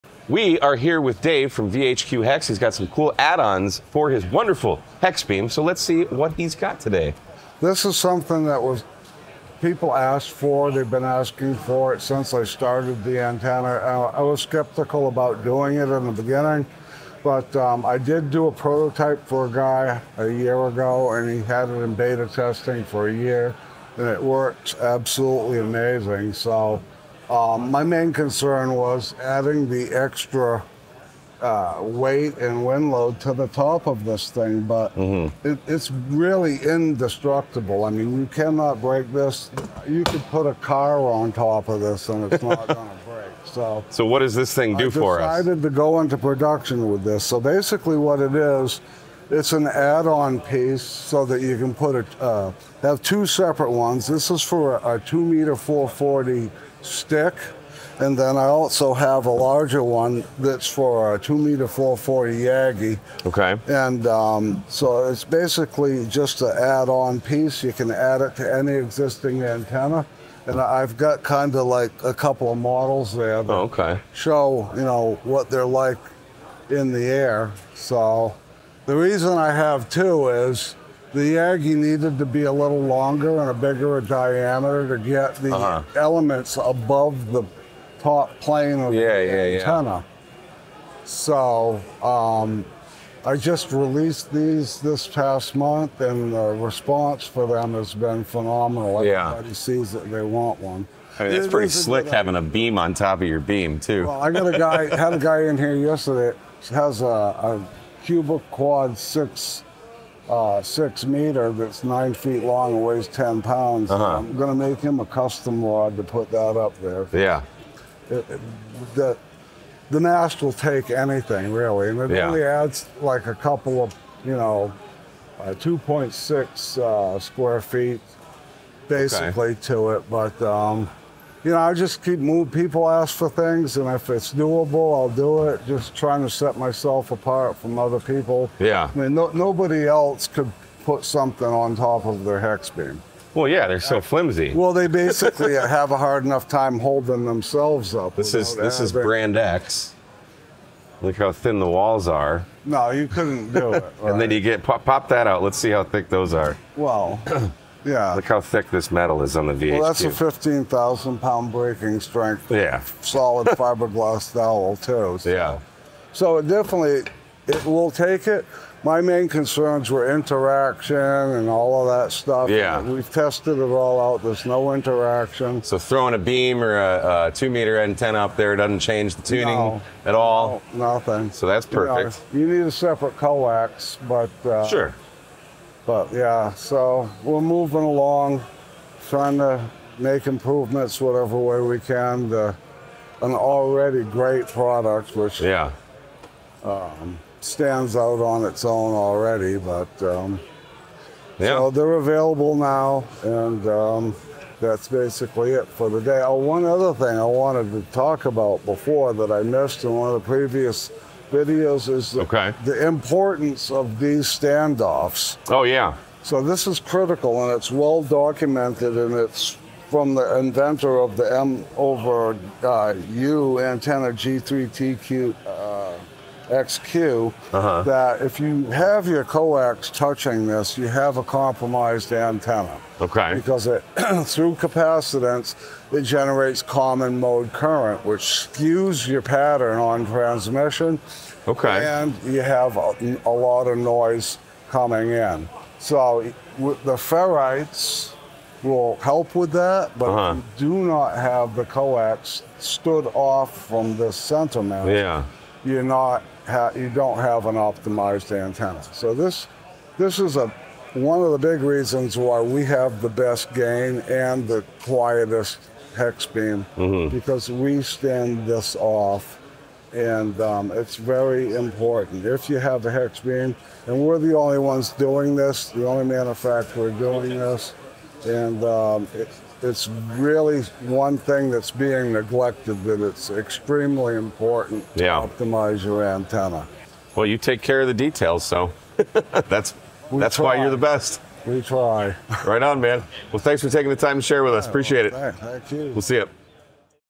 VHQ Hexbeams New Add On For VHF/UHF
Put A Vertical Or Yagi VHF/UHF Antenna On Top Of Your VHQ Hex Beam!